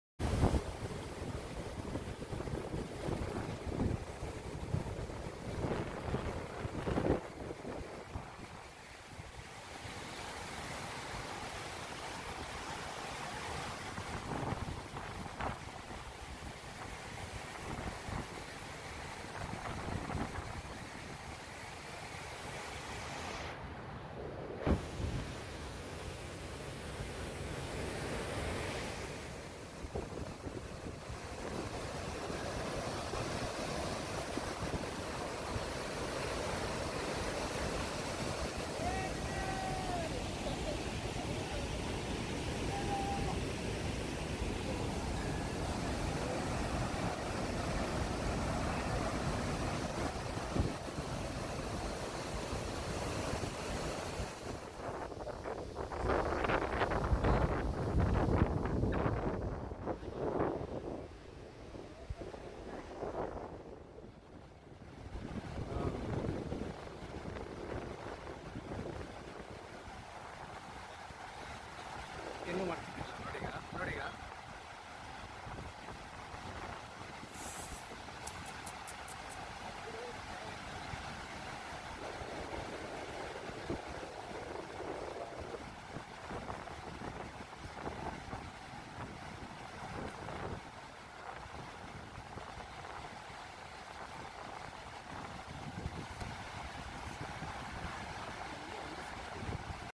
GOKAK FALLS .BELGAUM.
The Gokak Falls is a waterfall located on the Ghataprabha River in Belagavi district of Karnataka, India. The waterfall is six kilometers away from Gokak town.